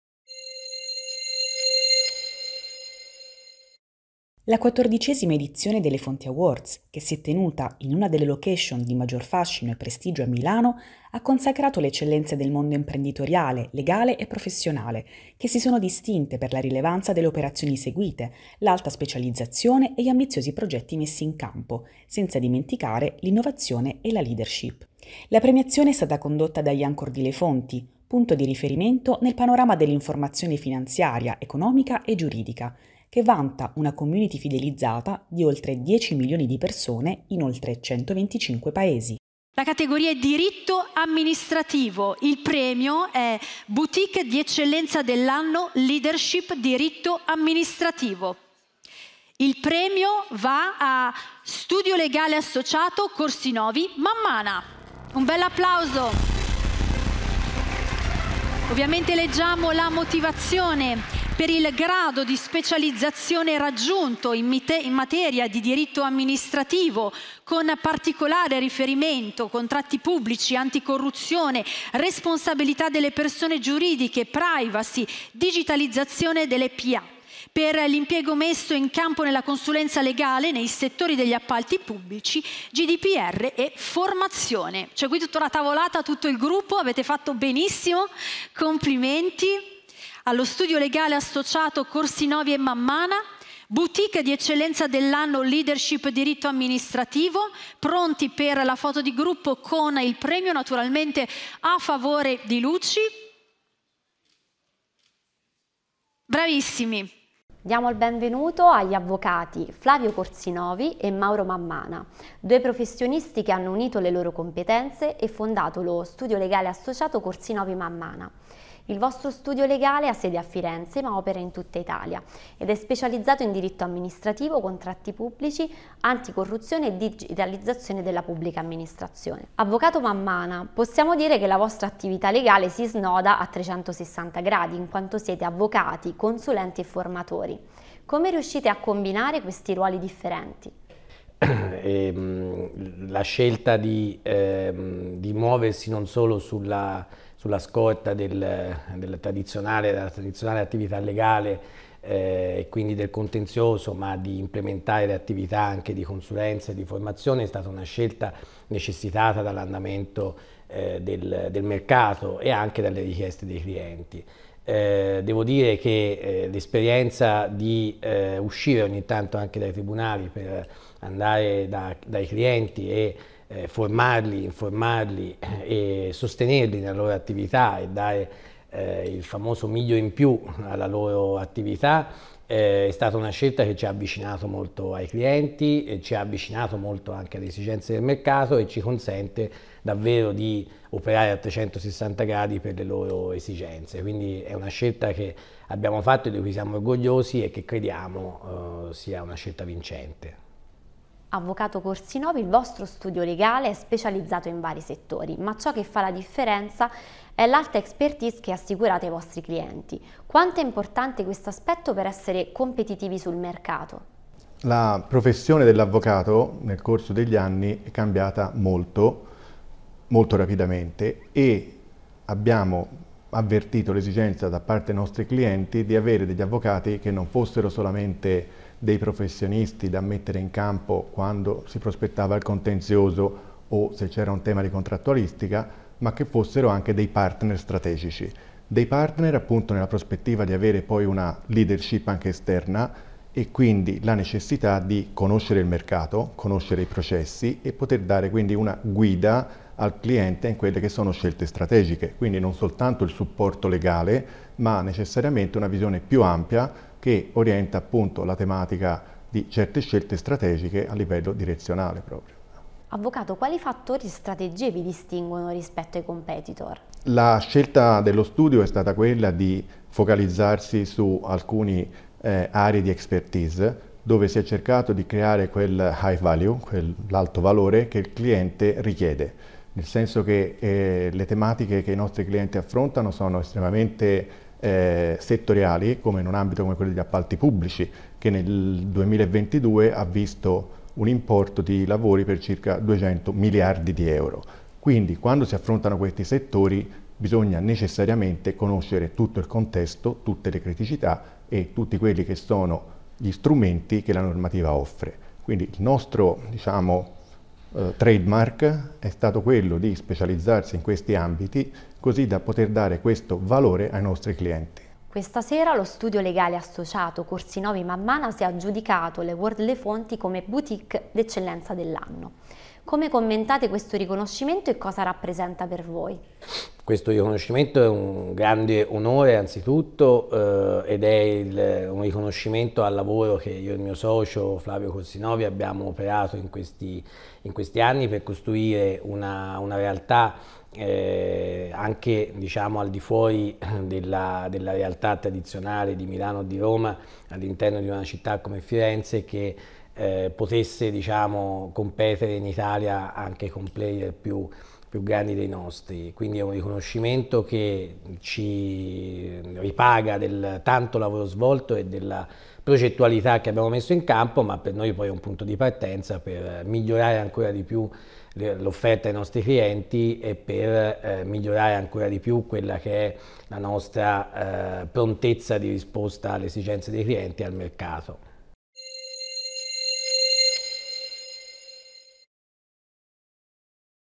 Studio Legale Corsinovi Mammana trionfa come Eccellenza dell’Anno, categoria Diritto Amministrativo
Studio Legale Associato Corsinovi Mammana trionfa come Boutique di Eccellenza dell’Anno Leadership, Diritto Amministrativo.

Per il grado di specializzazione raggiunto in materia di Diritto Amministrativo, con particolare riferimento contratti pubblici, anti corruzione, responsabilità delle persone giuridiche, privacy, digitalizzazione della PA. Per l’impegno messo in campo nella consulenza legale nei settori degli appalti pubblici, GDPR, formazione.

Intervista con Avv. Mauro Mammana (name partner) e Avv. Flavio Corsinovi (name partner)

👔 Le news per i CEO ed i CFO di successo

🎬 Tutte le notizie sul mondo della tv e del cinema